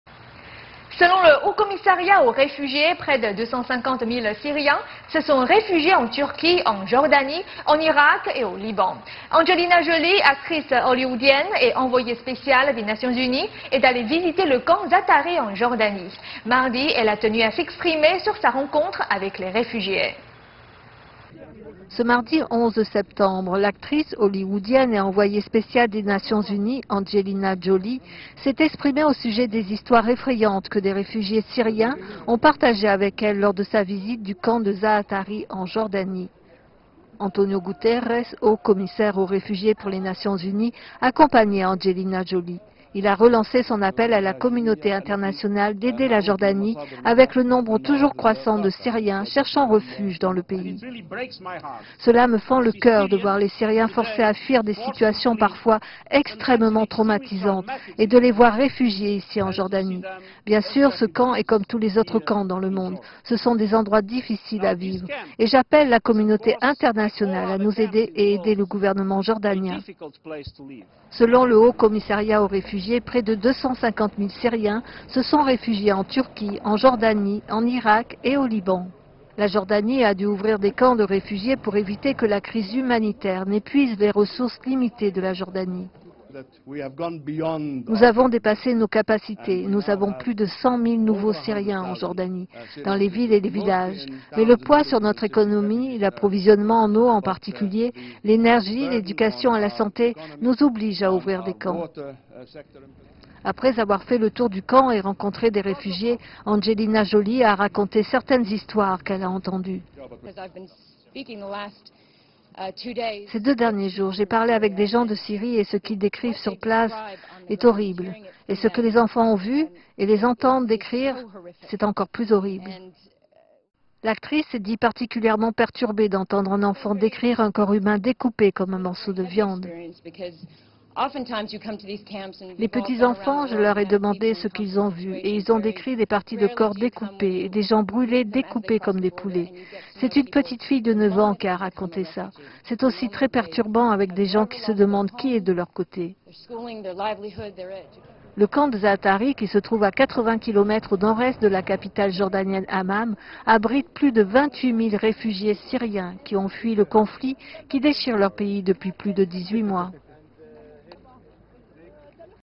Angelina Jolie visite le camp de réfugiés syriens de Zaatari
Selon le haut-commissariat aux réfugiés, près de 250 000 Syriens se sont réfugiés en Turquie, en Jordanie, en Irak et au Liban. L'actrice hollywoodienne et Envoyée spéciale des Nations Unies, Angelina Jolie, est allée visiter le camp Zaatari en Jordanie. Mardi, elle a tenu à s'exprimer sur sa rencontre avec les réfugiés.
Ce mardi 11 septembre, l'actrice holywoodienne et envoyée spéciale des Nations Unies Angelina Jolie s'est exprimée au sujet des histoires effrayantes que des réfugiés syriens ont partagées avec elle lors de sa visite du camp de Zaatari en Jordanie.

Antonio Guterres, haut commissaire aux réfugiés pour les Nations unies, accompagnait Angelina Jolie. Il a relancé son appel à la communauté internationale d'aider la Jordanie avec le nombre toujours croissant de Syriens cherchant refuge dans le pays.

[Antonio Guterres, Haut commissaire aux réfugiés pour les Nations unies]
«Cela me fend le cœur de voir les Syriens forcés à fuir des situations parfois extrêmement traumatisantes et de les voir réfugiés ici en Jordanie. Bien sûr, ce camp est comme tous les autres camps dans le monde, ce sont des endroits difficiles à vivre. Et j'appelle la communauté internationale à nous aider et aider le gouvernement jordanien.»

Selon le haut commissariat aux réfugiés, près de 250 000 Syriens se sont réfugiés en Turquie, en Jordanie, en Irak et au Liban.

La Jordanie a dû ouvrir des camps de réfugiés pour éviter que la crise humanitaire n'épuise les ressources limitées de la Jordanie.

[Naser Judeh, Ministre des Affaires étrangères, Jordanie]
«Nous avons dépassé nos capacités et nous avons plus de 100 000 nouveaux Syriens en Jordanie, dans les villes et les villages, mais le poids sur notre économie, l'approvisionnement en eau en particulier, l'énergie, l'éducation et la santé nous oblige à ouvrir des camps.»

Après avoir fait le tour du camp et rencontré des réfugiés, Angelina Jolie a raconté certaines histoires qu'elle a entendues.

[Angelina Jolie, Envoyée spéciale des Nations Unies pour les réfugiés]
«Ces deux derniers jours, j'ai parlé avec des gens de Syrie et ce qu'ils décrivent sur place et entendre cela est horrible et ce que les enfants ont vu et les entendre décrire cela.»

L'actrice s'est dit particulièrement perturbée d'entendre un enfant décrire un corps humain découpé comme un morceau de viande.

«Les petits enfants, je leur ai demandé ce qu'ils ont vu et ils ont décrit des parties de corps découpées et des gens brûlés découpés comme des poulets. C'est une petite fille de neuf ans qui a raconté ça. C'est aussi très perturbant avec des gens qui se demandent qui est de leur côté.»

Le camp de Zaatari, 80 kilomètres au nord-est de la capitale jordanienne Amman, abrite plus de 28 000 réfugiés syriens qui ont fui le conflit qui déchire leur pays depuis plus de 18 mois.